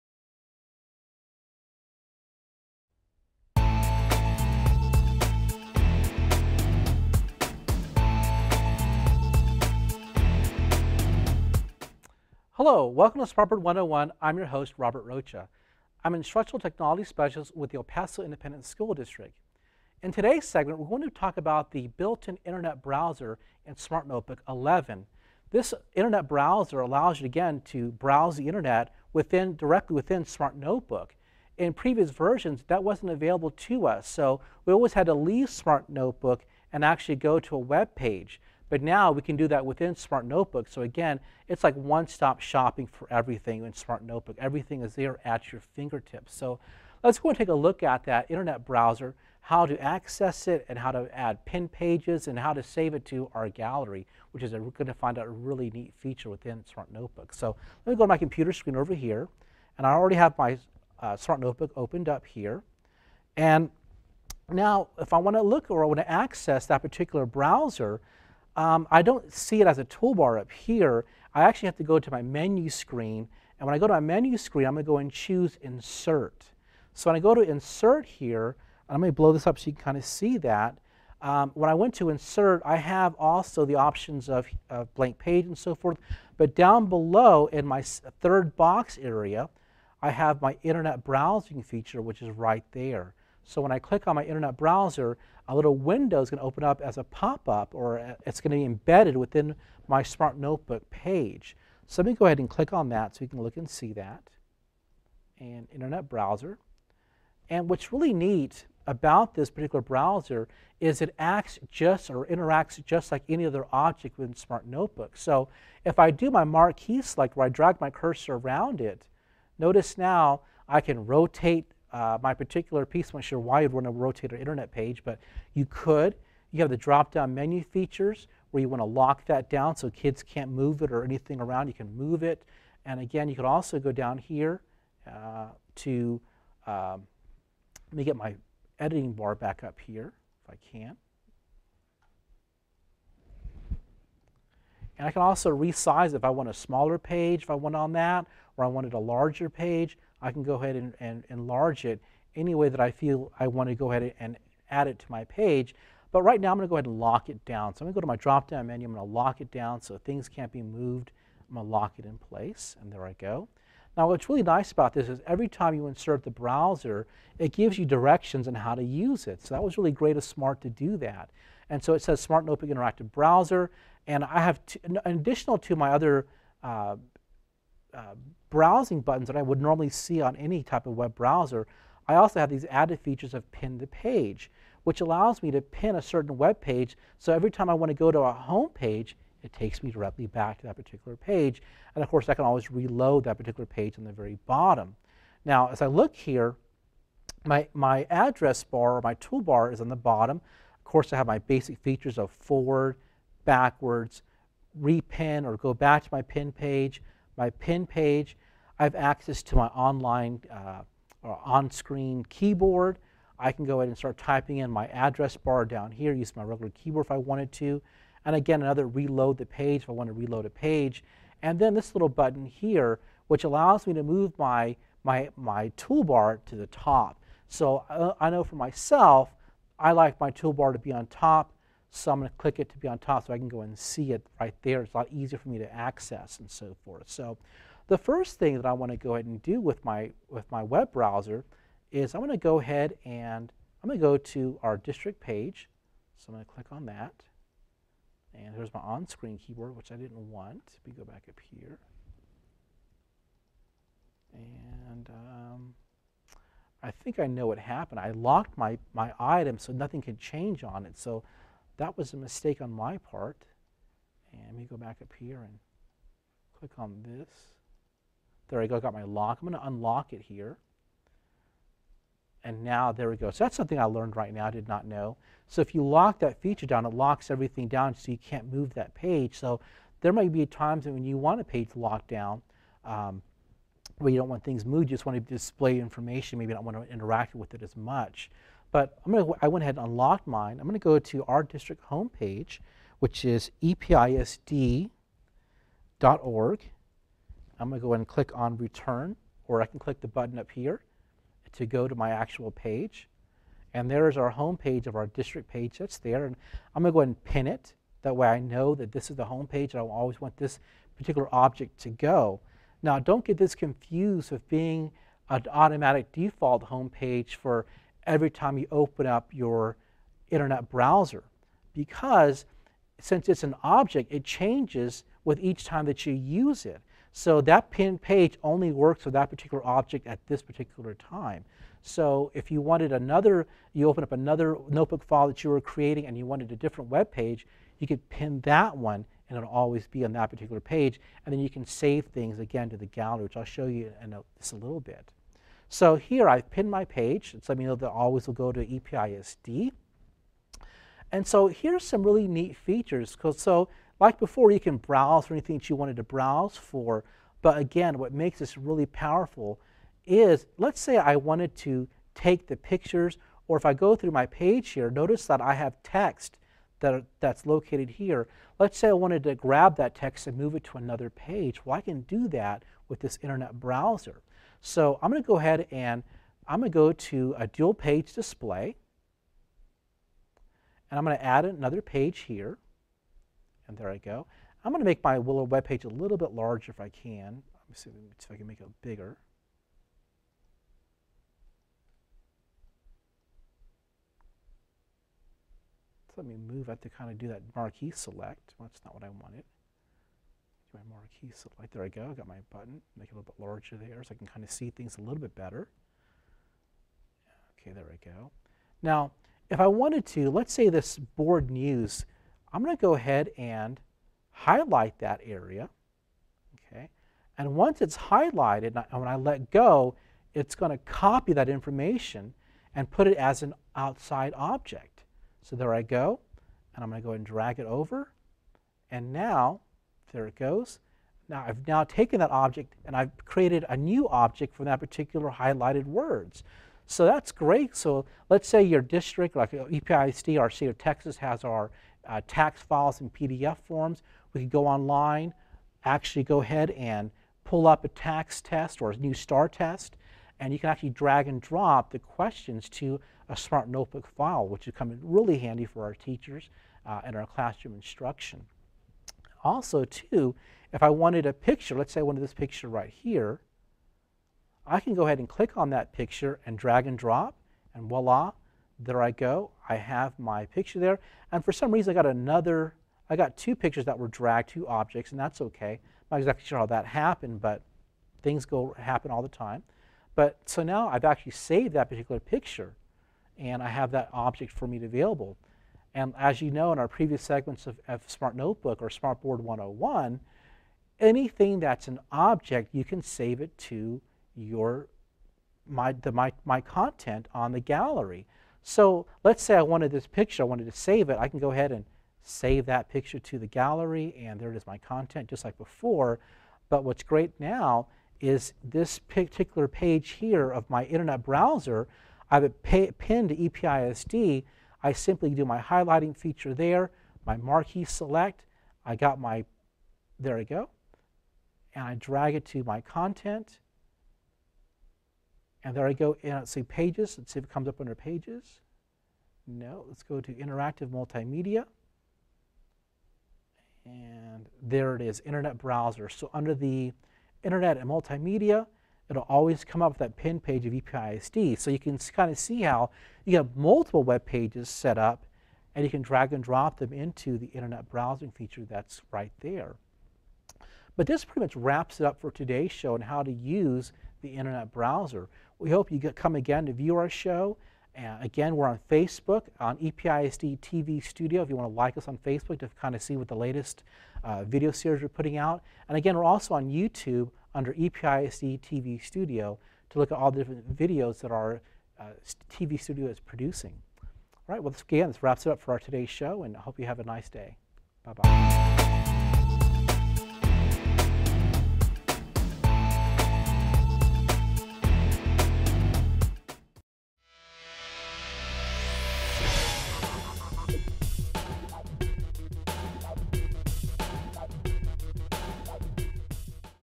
Smart Board 101 - Web Browser
This segment covers the new Web Browser option in Smart Notebook 11.  How to use it, and how it can be included into your lesson without exiting or changing windows.